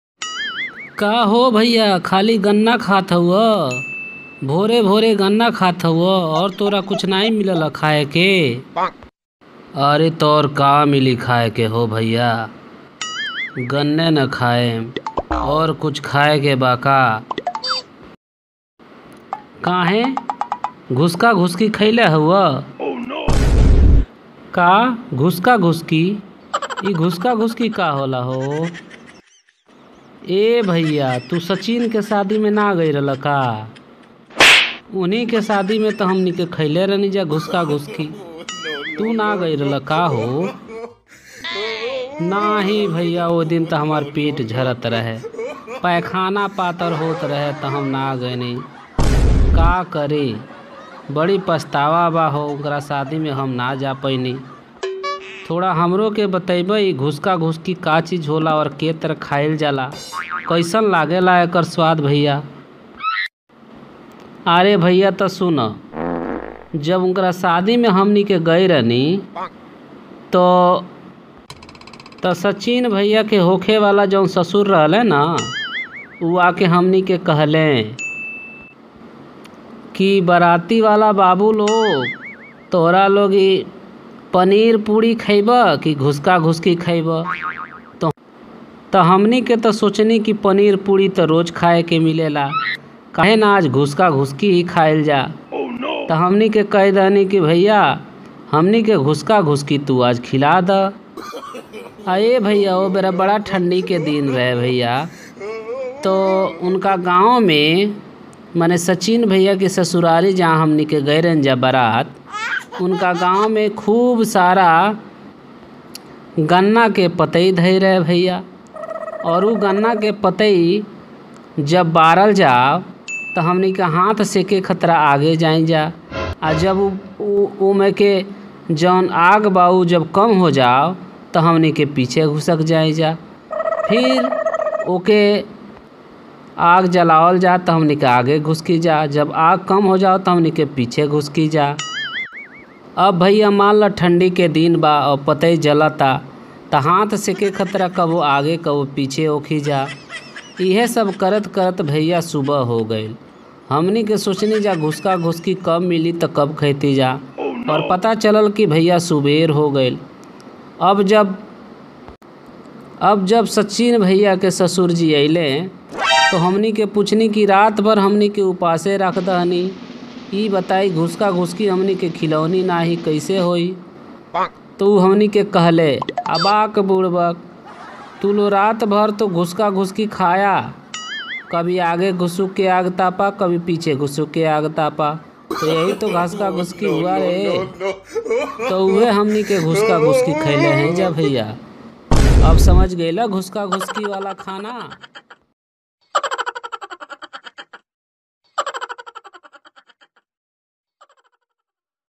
बारात में खाने को मिला 'घुसका - घुसकी' 🤪😀😀😀😀😀 Funny cricketers dubbing video
बारात में मिला खाने को घुसका - घुसकी 🤪😀😀😀😀😀  Funny cricketers dubbing video
क्रिकेटर्स डबिंग फनी विडियो